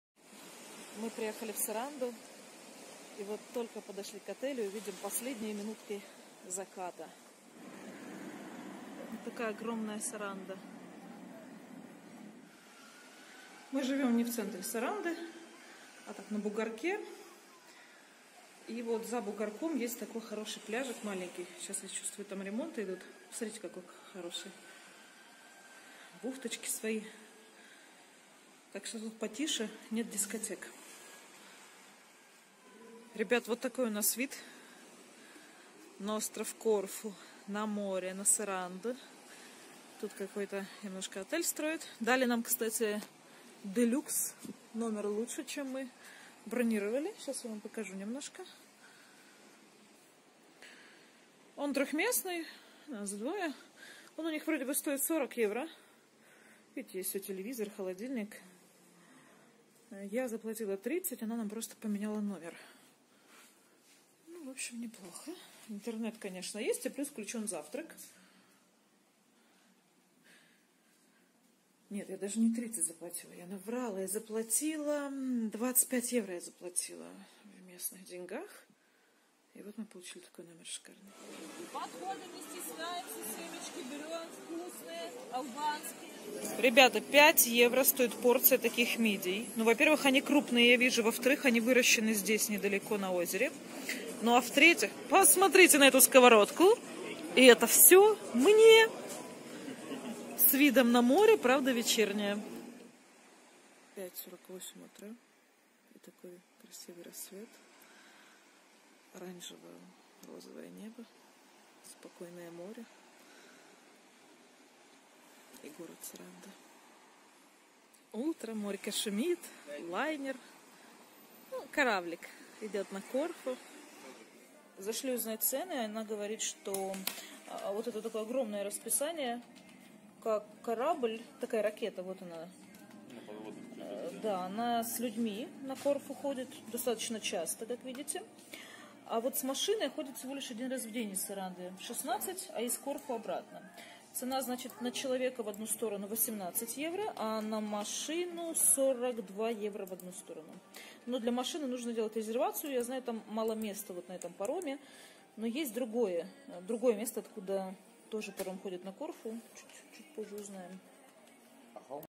Саранда - главный курорт на юге Албании 🌴
Прекрасные бухточки Саранды и пляжи вокруг города, широкая набережная, еще невысокие цены - вот, чем привлекает этот курорт Албании. Огромный город, чем -то напомнивший мне Ялту и Одессу.  Мы были 11-12 мая в несезон.